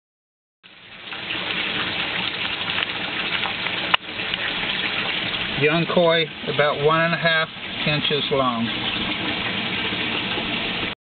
Young Koi
These Koi are about 2 inches on average. As of January 2013 I have very low inventory of small Koi. Later this summer there will be a large supply available. Check back in May.